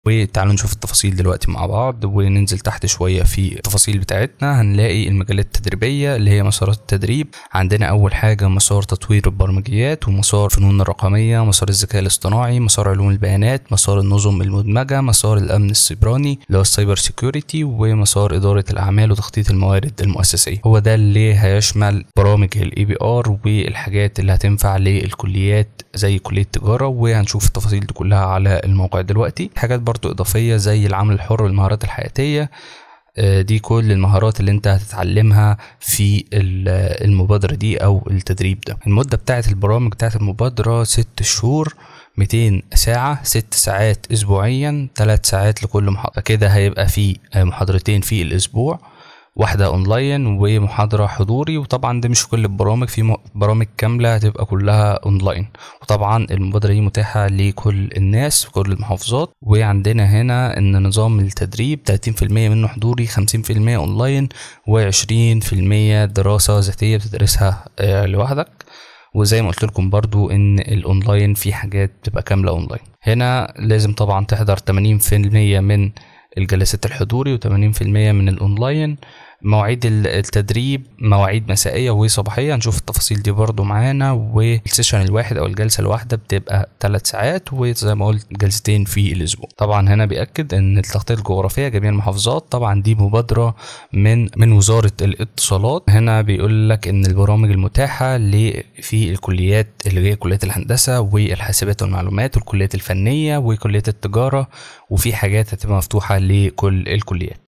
تفاصيل المنحة المجانية مبادرة رواد مصر الرقمية من وزارة الاتصالات وتكنولوجيا المعلومات DEPI
☑️ إدعم القناة عبر مشاركة الفيديو و تشغيل التنبيهات (🔔)

🧭 جدول المحتويات

🎥 فيديوهات قد تهمك

🎬 قوائم تشغيل قد تهمك
شرح التقديم للتدريبات